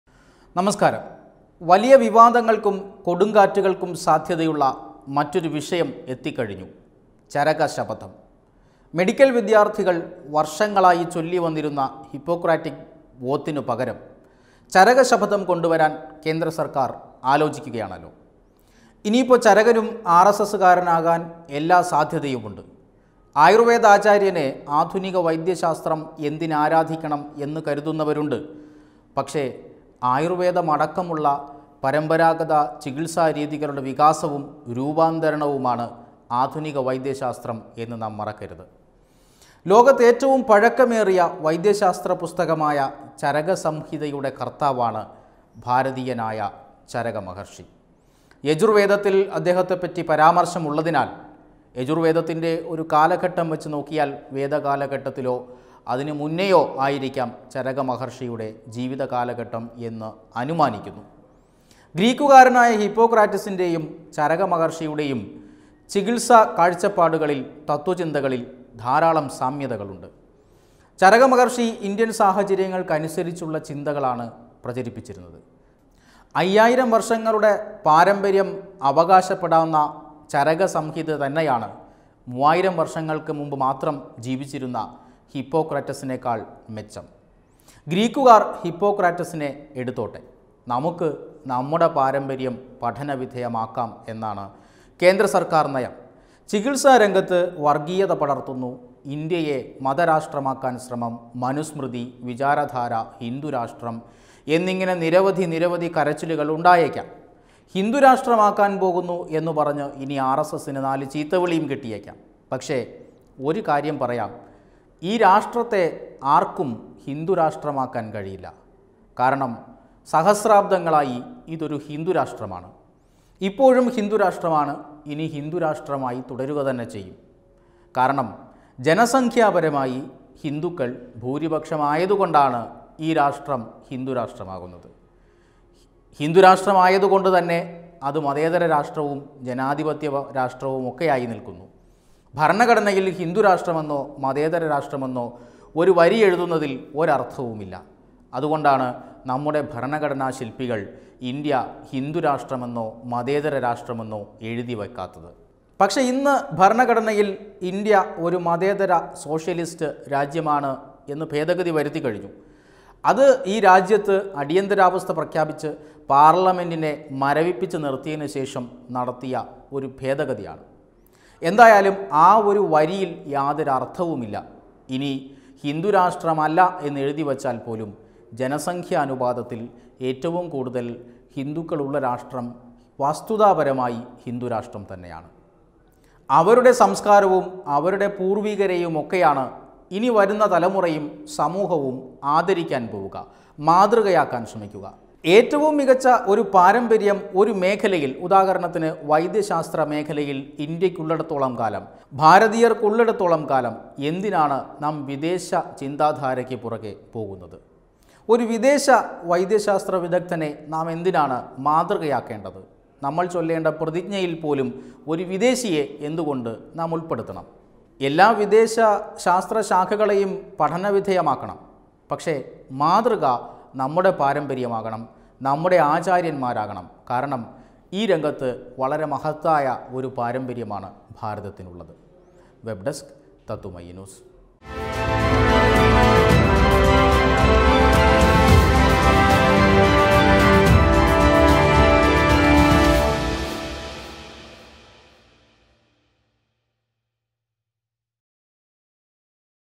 ഹിപ്പോക്രാറ്റിക് പ്രതിജ്ഞ മാറി ചാരക ശപഥം വരുമ്പോൾ I HIPPOCRATIC OATH
ചരക ശപഥം കേട്ട് വിറളി പിടിക്കാൻ വരട്ടെ രാജ്യം തിരുത്തുന്നത് പതിറ്റാണ്ടുകളുടെ തെറ്റ്